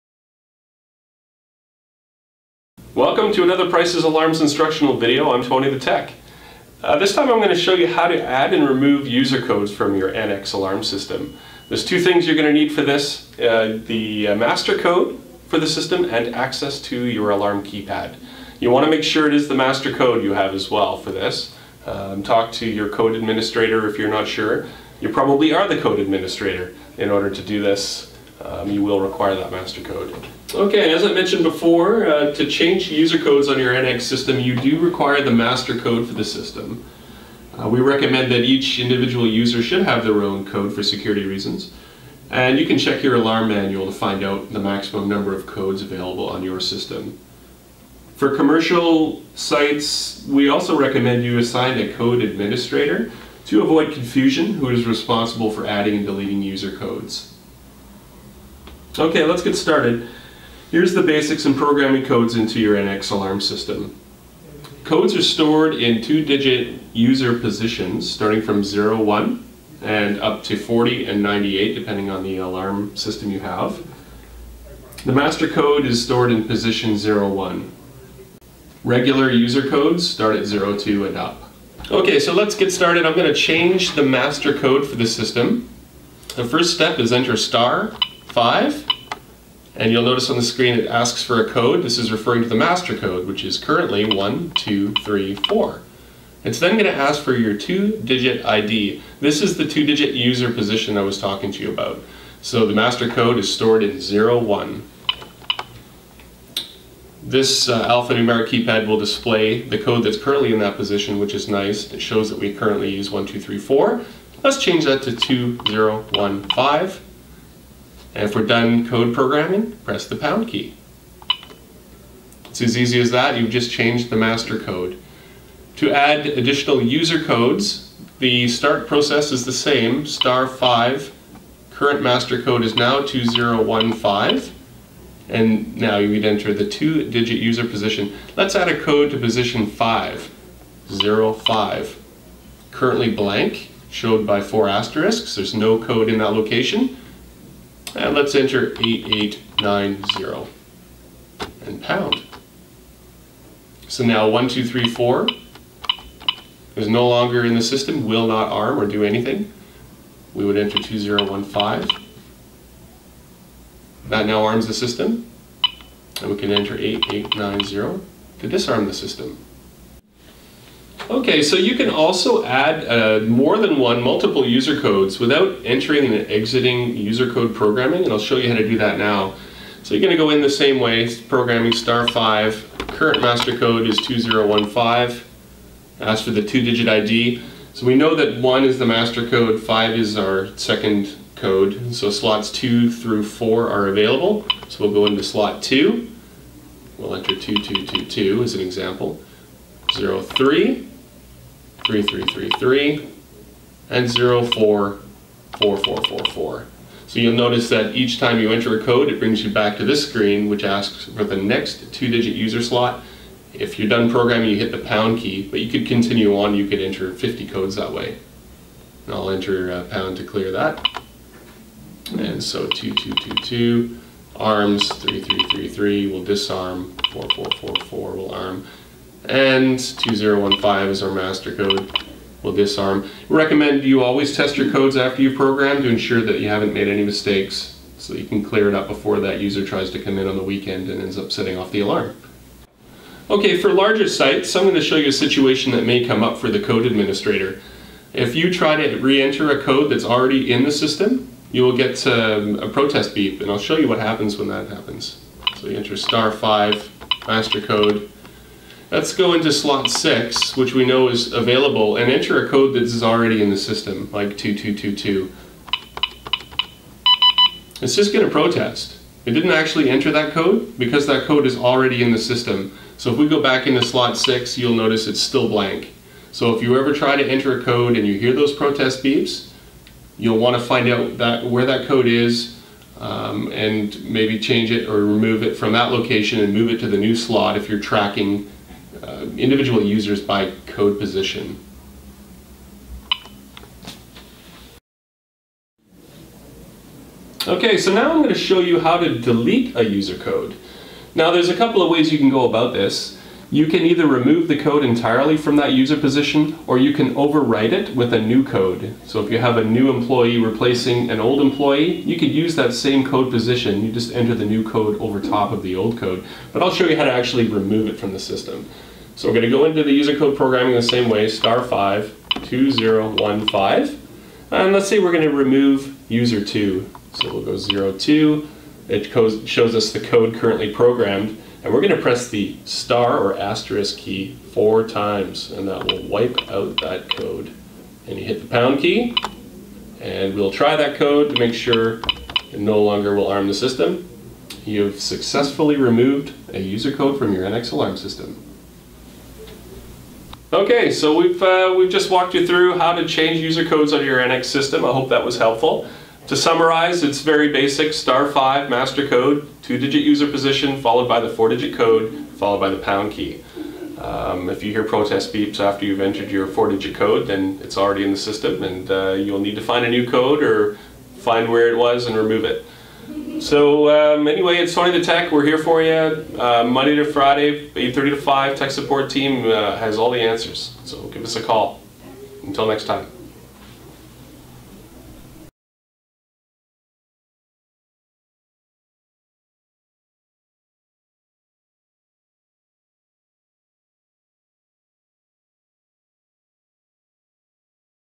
Adding & Deleting Alarm Codes on GE / Interlogix NX Security System
Tony the Tech from Price's Alarms walks us through the process of adding and deleting alarm codes on a GE / Interlogix NX Security System